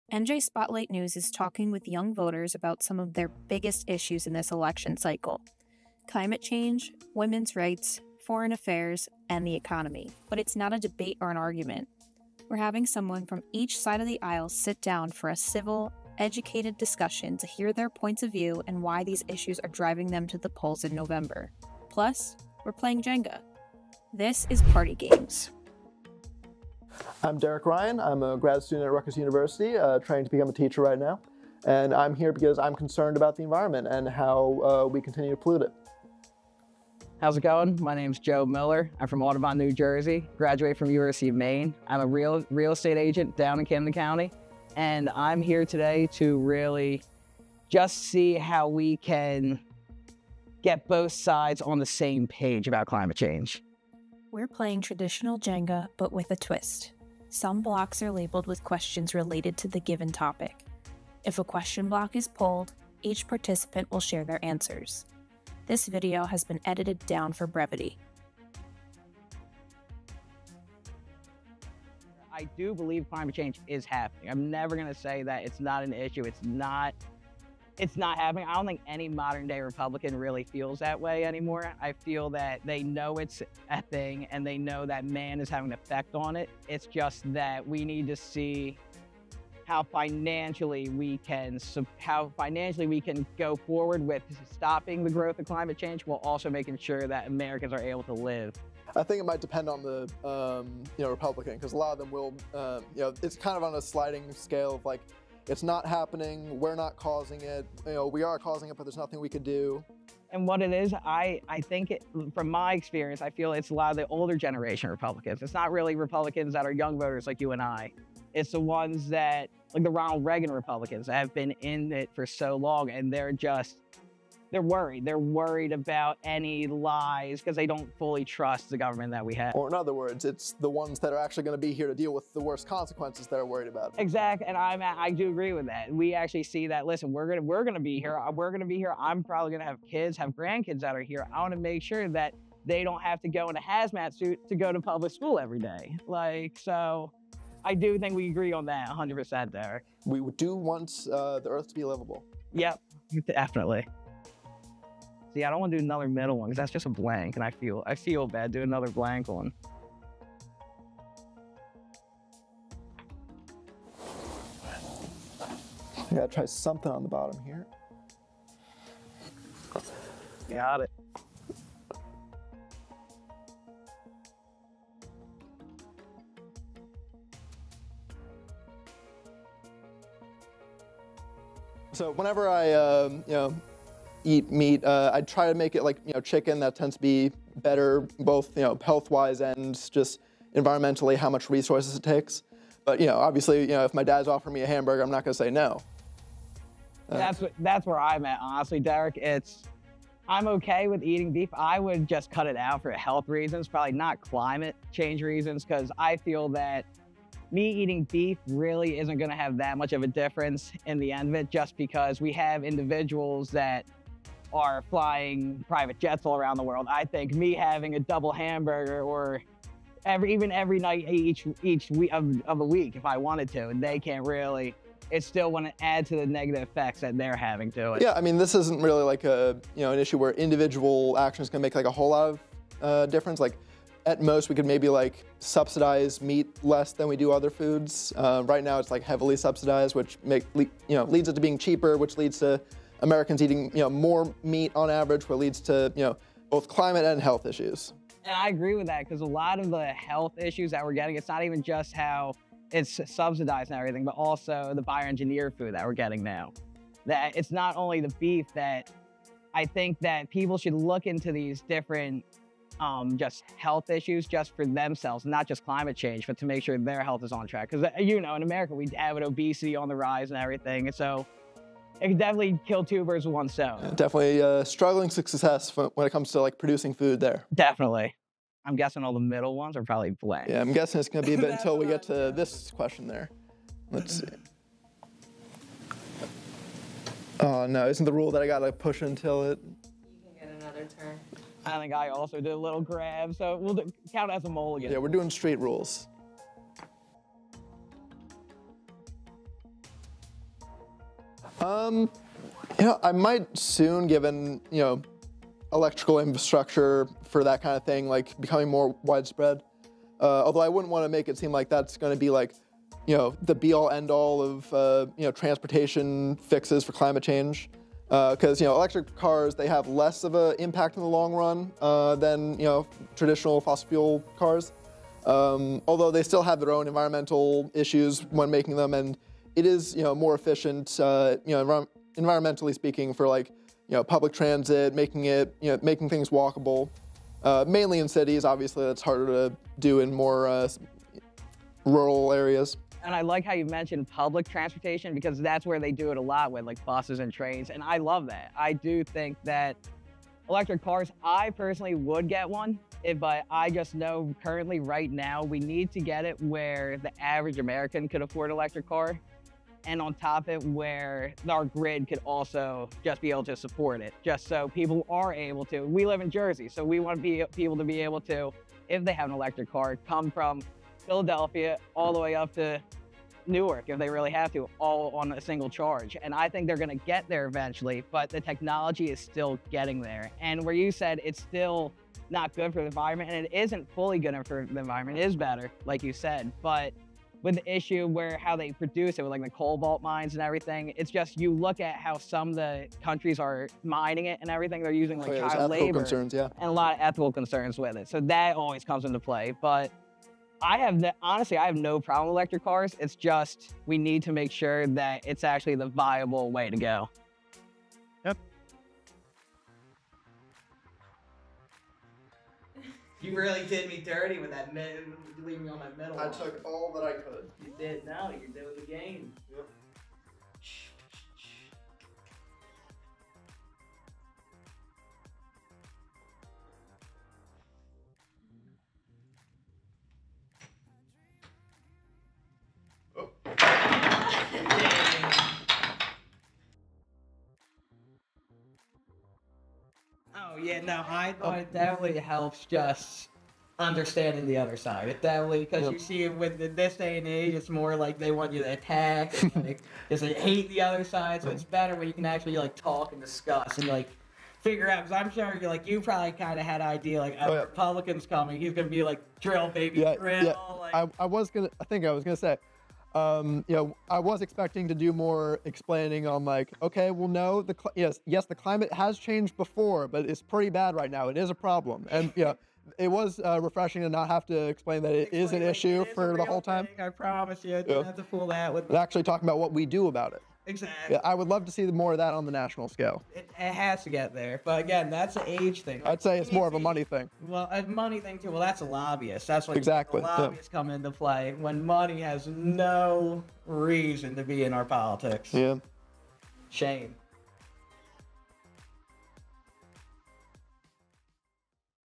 A conversation on climate change: Young voters discuss top issues ahead of November election
The 2024 election is just a few weeks away, and climate change is one of the biggest issues for young voters. NJ residents Derek Ryan and Joe Miller sat down for a civil conversation to share their different points of view and discuss why climate change is driving each of them to the polls on Nov. 5.

00:00 Intro
1:10 Does climate change exist?
2:54 Have you ever considered cutting out beef or other foods because of climate concerns?
4:58 How do you feel about electric cars?
7:17 How'd the conversation go?